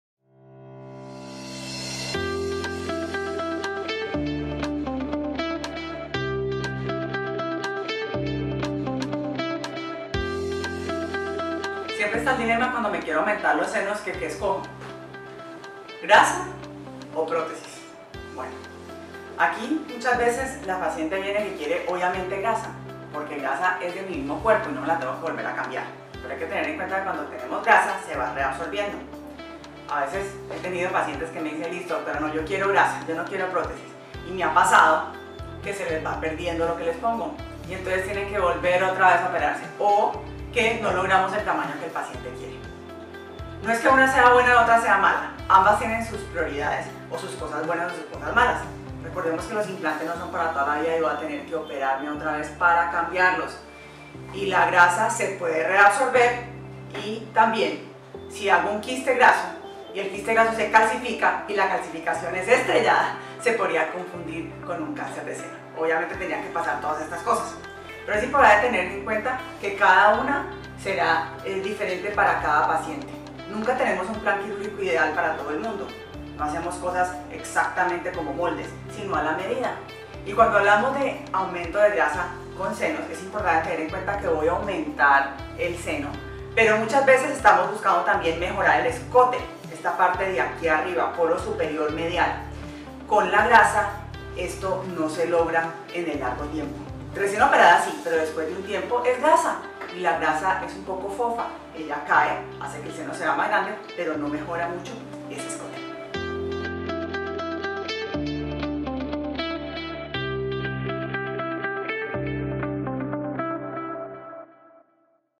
Aumento de senos con grasa o prótesis
Aumento de senos con grasa o con prótesis. Esta es una duda frecuente con la que llegan mis paciente a consultA. Las dos son buenas alternativas por evaluaré contigo los pros y contras de cada una de ellas, y de acuerdo a tus necesidades elaboraremos un plan quirúrgico pensado en ti.
Breast augmentation with fat or implants. This is a frequent question my patients have during consultation. Both are good alternatives. During assessment I will evaluate with you the pros and cons of each of them, and according to what you want and your needs we will develop a surgical plan designed for you.